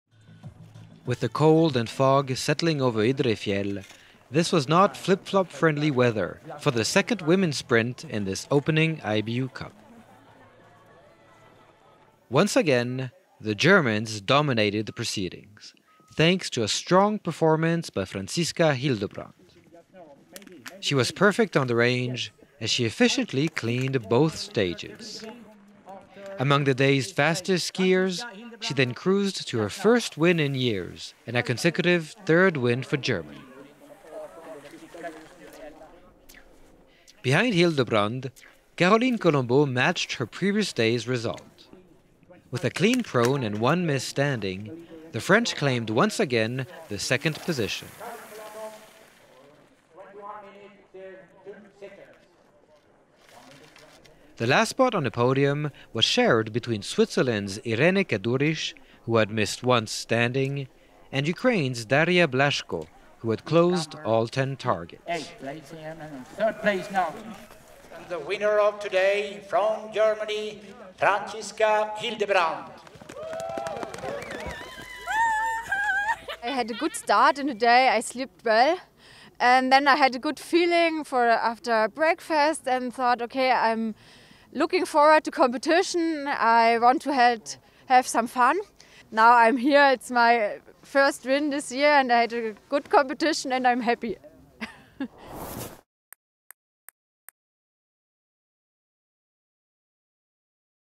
IBU Cup 21/22 Idre Women Sprint 2 Highlights
Continuing their winning streak at the start of  this IBU Cup season,
the Germans made it three victories in three competitions, thanks to a strong performance by Franziska Hildebrand in the second Women Sprint in Idre Fjäll.